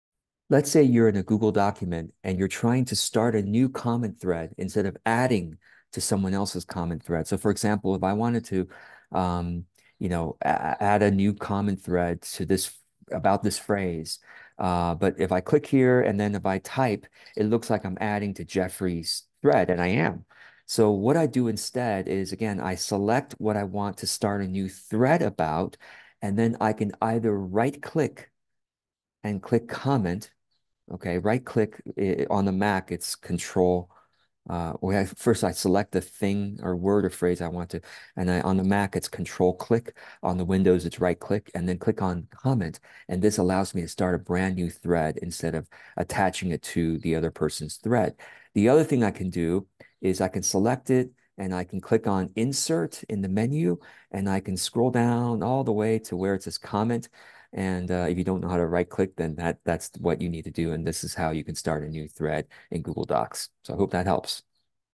Google Doc: how to start a NEW comment thread instead of replying to someone's existing comment
Steps:

1. Select the word/phrase you want to start a new comment thread with.
2. Click "Insert" in the menu, then "Comment"
3. Type and submit your comment!